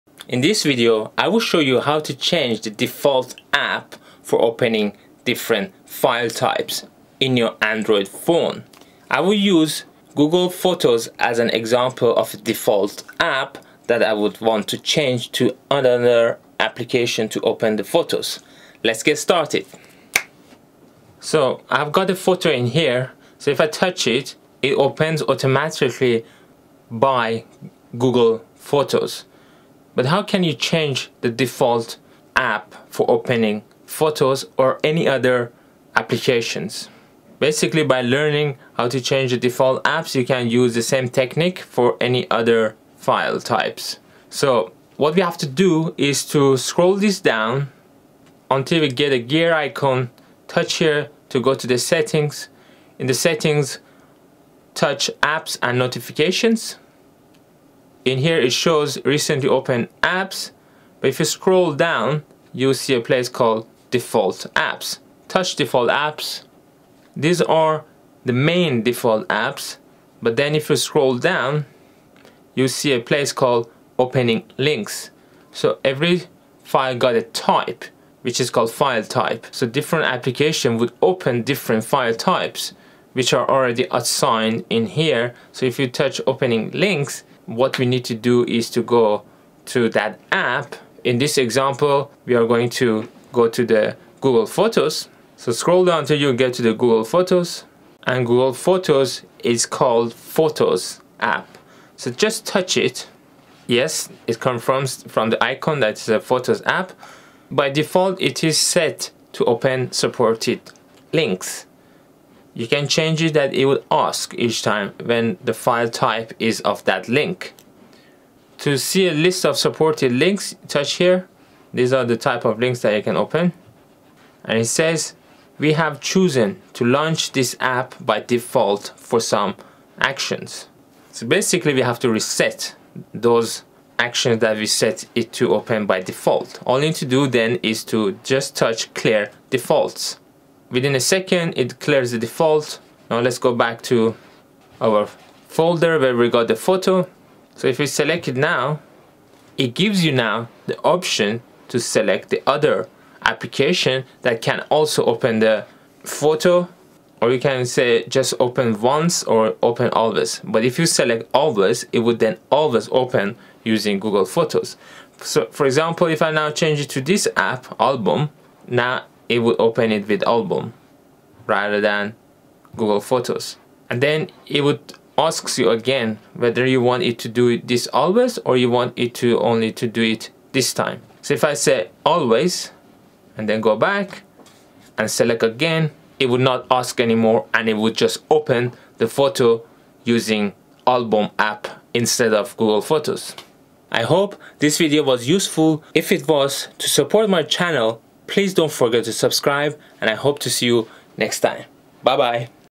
how to change the DEFAULT app on android
In this video, I'll show you set by step by how change the default app. In android, once you setup the default, it wouldn't be easy to change it. For example, you might notice that some photos are opened automatically with the google photos app, but you want to change it to other apps such as album, sketch, etc. So, you could use the same technique to the default apps for any app such playing music, messaging, etc.